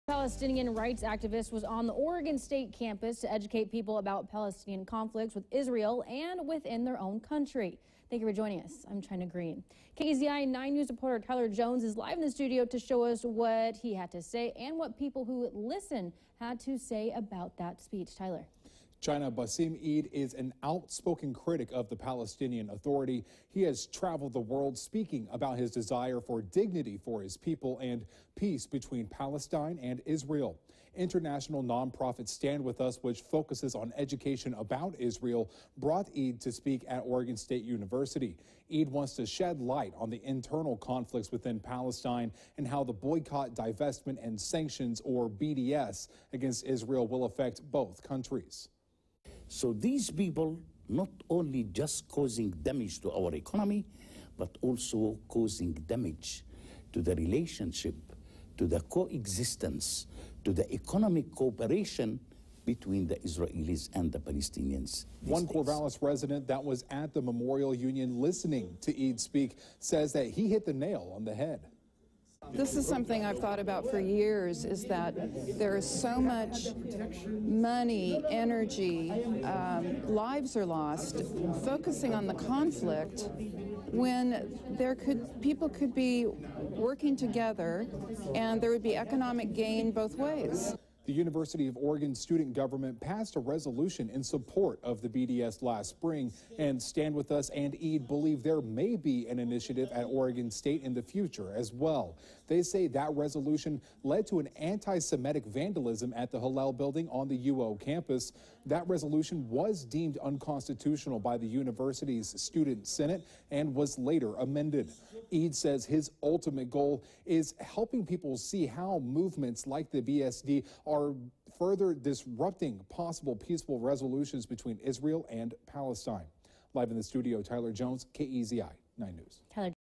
Palestinian human rights activist speaks at Oregon State
Bassem Eid is an outspoken critic of the Palestinian Authority and has traveled the world speaking about his desire for dignity for his people and peace between Palestine and Israel.